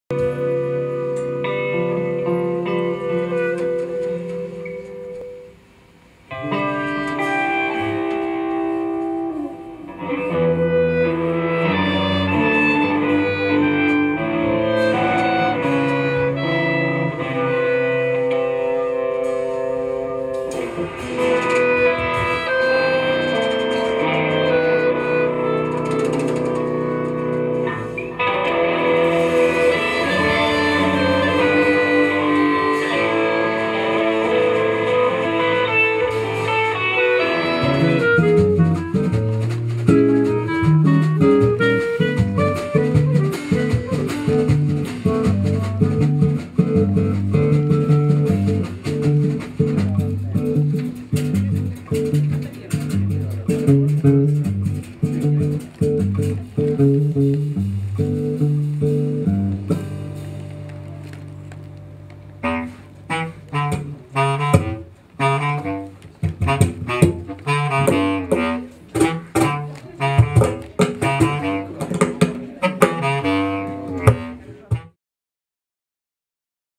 Cinema - Concert a Valldoreix
cine-concert amb "La ley de la hospitalidad" de Buster Keaton. Acompanyaran la pel·lícula en directe Barolomeo Barenghi a la guitarra, Pau Domènech al clarinet, flauta i saxo i Josema Martín a la bateria.